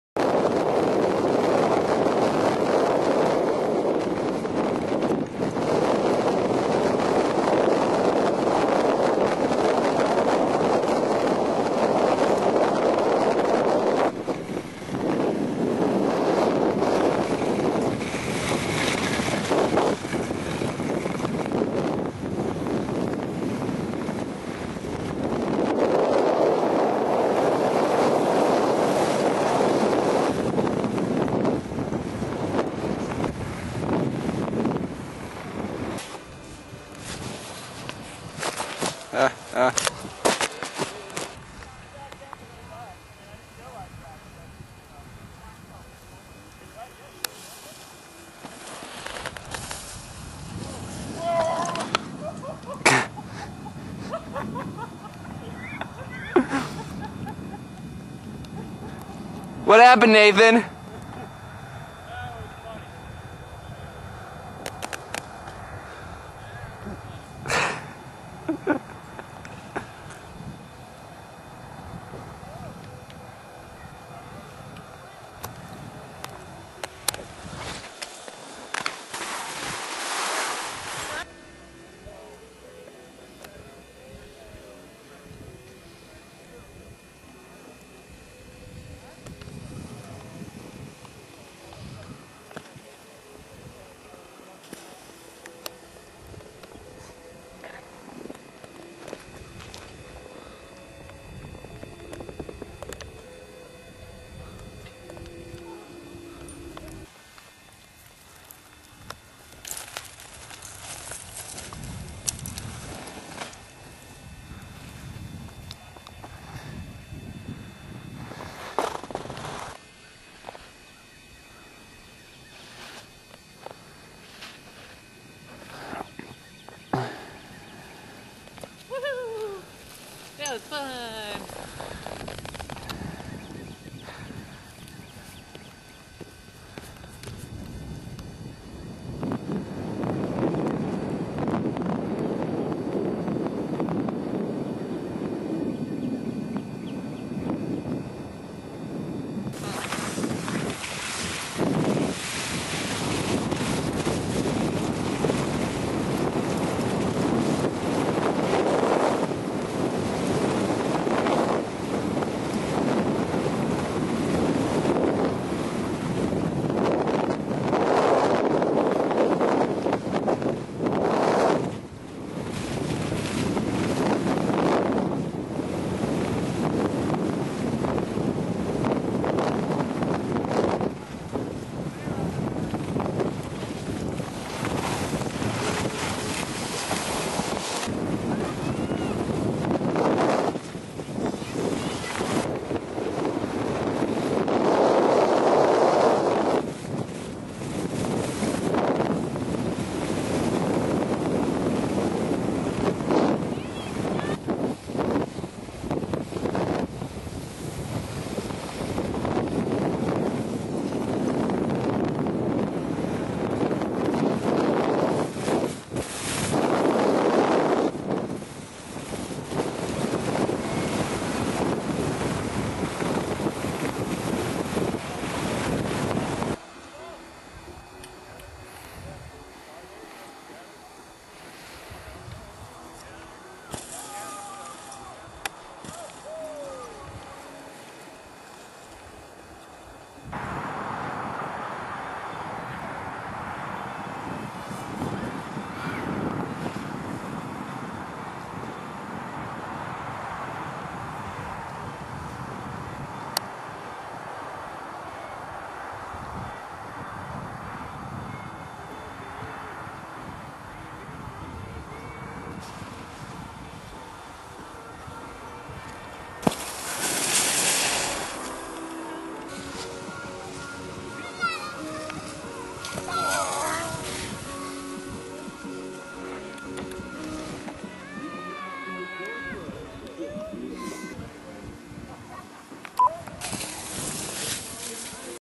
Skiing at Holy Hill, The next week
You'll have to bear with us for the first part of the video. We tried taping each other while the person with the camera was also skiing. let's just say you might want to find some Dramamine before you watch this. Anywho, the good stuff is near the end. Enjoy!